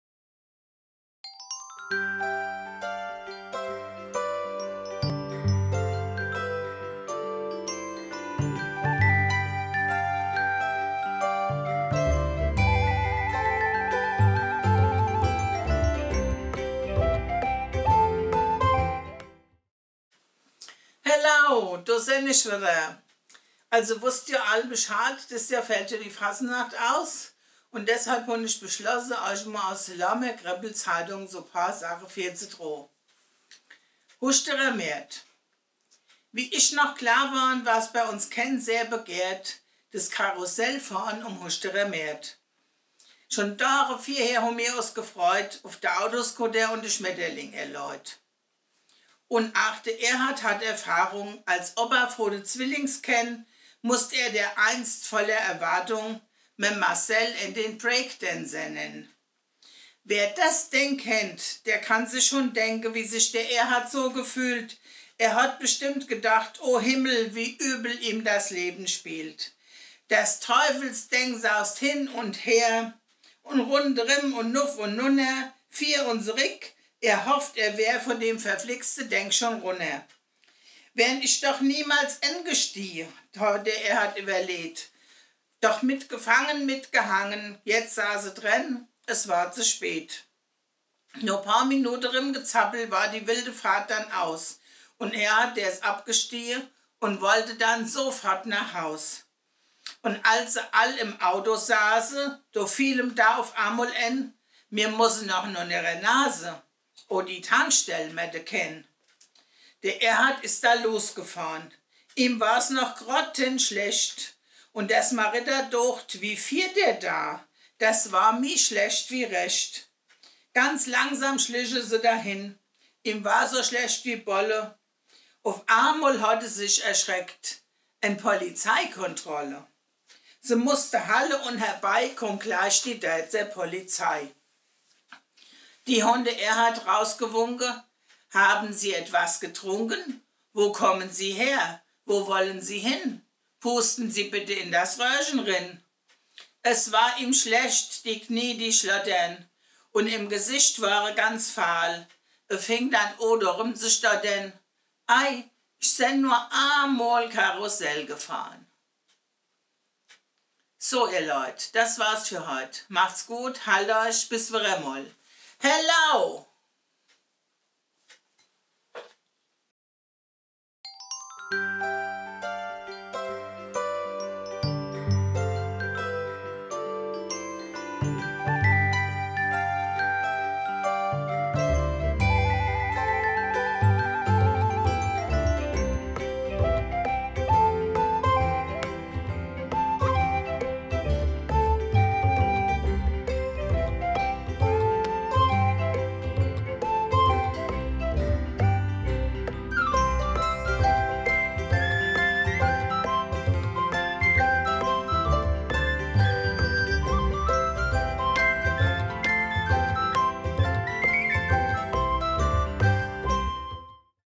Huustirrer Mert/Hahnstätter Markt-Best of Lohrmer Kreppelzeitung
Helau!

In diesem Jahr fällt ja leider die Fassenacht in der üblichen Form aus, aber ich finde, man kann auch zuhause mit seinen Lieben feiern und sich auch an lange zurück liegende lustige Begebenheiten erinnern.
1985 habe ich damit begonnen, für die Lohrheimer Fassenacht die sogenannte Kreppelzeitung zu schreiben, wo alle Neuigkeiten und Missgeschicke, die in unserem Dorf passiert sind aufgelistet und an der Fassenacht durch den Kakao gezogen werden.
Zwischen 2005 und 2017 gabs dann für mich einen längere Faschingspause, aber danach ging es wieder weiter. So entstanden viele kleine Anekdoten und Sketches, die zur allgemeinen Erheiterung beitrugen und so manchem Fassenachts-Besucher einige unbeschwerte frohe Stunden beschert haben. Lange Jahre habe ich die Kreppelzeitung gemeinsam mit meiner Freundin Ingrid, mit Liesel und dann auch mit Christine und neuerdings mit Christiane vorgetragen. Wir traten gemeinsam unter anderem als "Jettche un Settche", als Chip und Chap, als kleine Hexen, als Musketiere, als Speiß-Girls, als Gartenzwerge und als Piraten auf, vielleicht erinnern sich einige ja noch daran.
In all den Jahren ist eine wahrer Schatz an lustigen Begebenheiten und Geschichten über Missgeschicke zusammengekommen, aus dem ich gerne einige Highlights aus dem Archiv der Lohrheimer Fassenacht vortragen möchte, um die Fassenachts - lose Zeit damit zu überbrücken.

Viel Spaß damit. Ich rufe euch ein dreifach donnerndes Helau Helau helau!

's Christina vo Lohrm